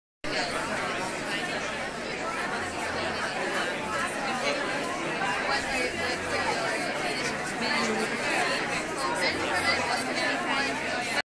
Toronto Go Global Student Fair
Over 5,000 students visited the event. Great interest for Switzerland&apos;s universities.